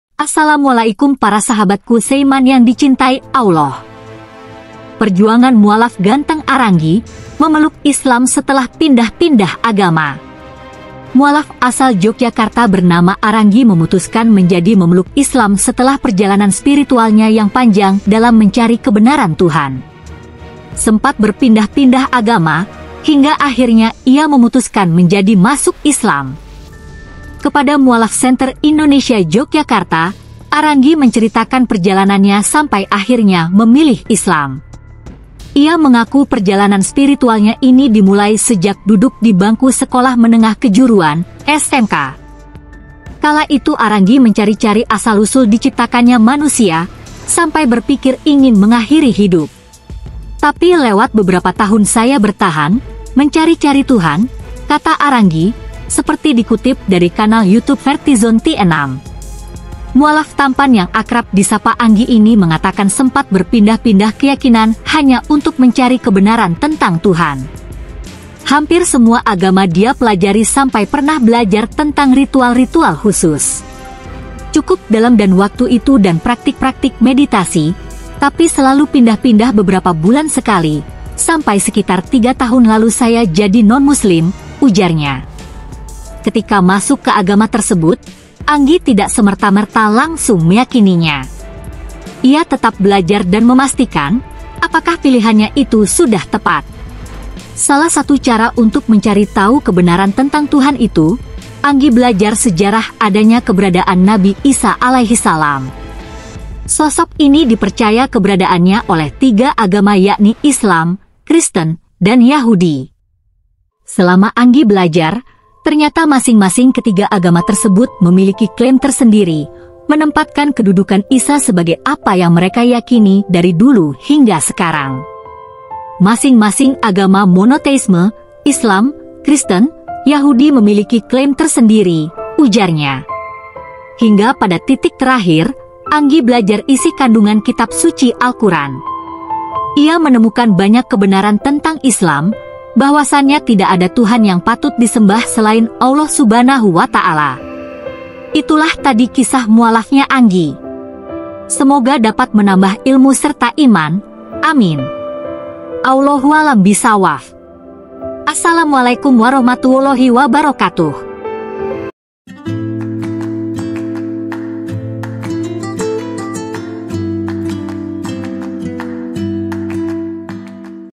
Kisah Mualaf Aranggi, Mantap Bersyahadat Setelah Mencicipi Semua Agama
Pada video ini menceritakan Kisah Mualaf Aranggi, Mantap Bersyahadat Setelah Mencicipi Semua Agama. Inilah kisahnya!